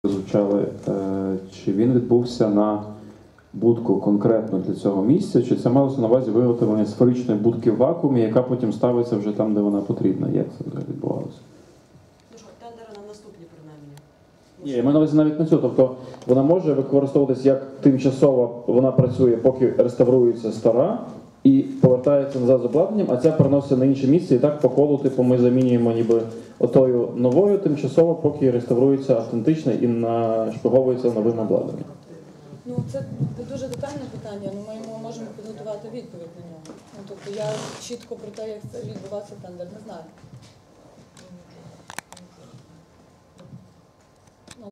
Візія Шутюка про послідовну заміну КТП
Представник ЛКП "Інститут просторового розвитку" Олександр Шутюк на гродмаському обговоренні заміни КТП.